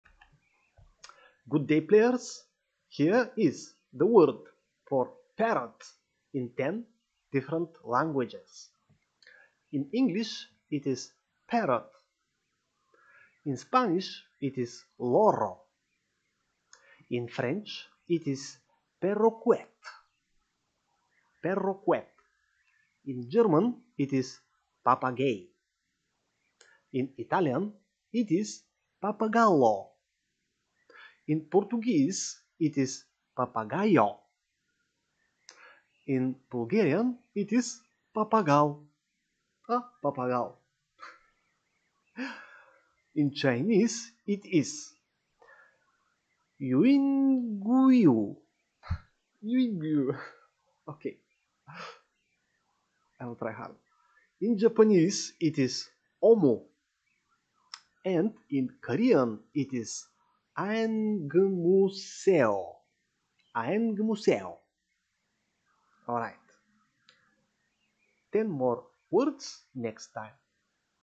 Pronouncing Parrot in 10 Languages 🎓
What is the word for Parrot in 10 different languages.